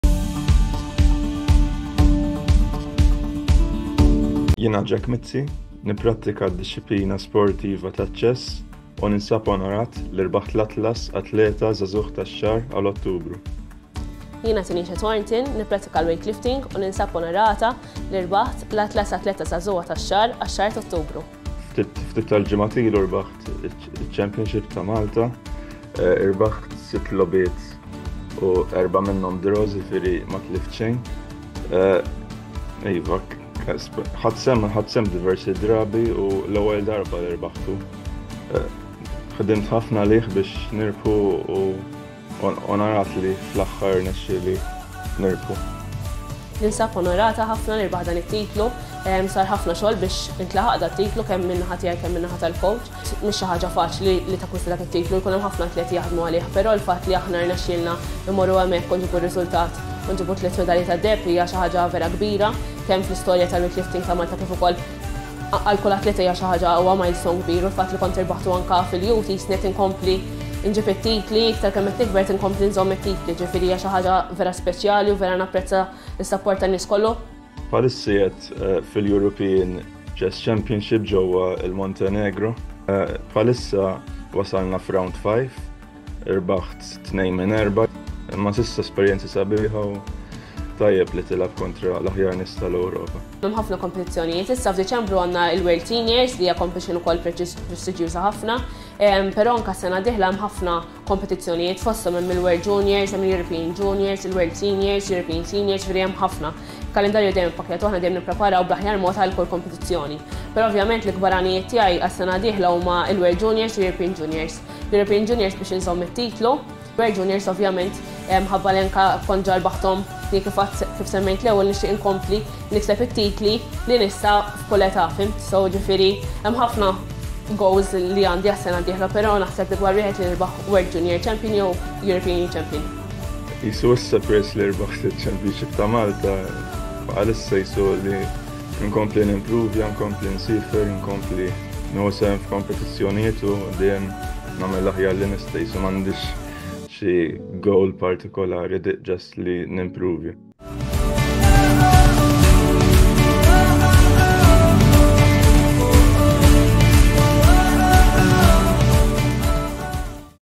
Atlas Youth Athlete of the Month Award - October 2024
Jack Mizzi (Chess) and Tenishia Thornton (Weightlifting) are the winners of the Atlas Youth Athlete of the Month Award for October.
Jack Mizzi who is 18 years old, has won the 89th edition of the Malta Chess Championship and re-confirmed the Junior Champion title for the third consecutive year.
In the Final with a double round robin format, finalists played twice against each other, once with the white pieces and once with the black pieces. Mizzi was the only unbeaten player achieving 8 points out of the 10 maximum points. Mizzi is one of the youngest winners of the Malta Chess Championship.
Meanwhile, Tenishia Thornton, aged 19, was crowned as the new European Champion at the European Junior Championships in Warsaw, Poland.
Thornton kicked off the Snatch portion with an impressive 86 kilogrammes lift and finally made a successful attempt at 89 kilogrammes. In the Clean and Jerk, Thornton started strongly at 107 kilogrammes, as she aimed for 110 kilogrammes, only to falter on the jerk. In her final dramatic attempt, she summoned every ounce of strength and determination, smashing the bar with a spectacular lift of 112 kilogrammes! Tenishia Thornton not only secured her spot at the top but also set a new national record. With a total lift of 201 kilogrammes, Thornton outpaced Spain who finished with 194 kilogrammes and Romania at 196 kilogrammes.
The Atlas Youth Athlete of the Month Award was presented to Tenishia Thornton by Matthew von Brockdorff, Managing Director and CEO of Atlas Insurance. The Malta Sports Journalists Association (MSJA) was represented by President Clinton Buhagiar and Administrator Stephen Azzopardi. Jack Mizzi’s presentation will be done at a later stage when he’ll be back in Malta.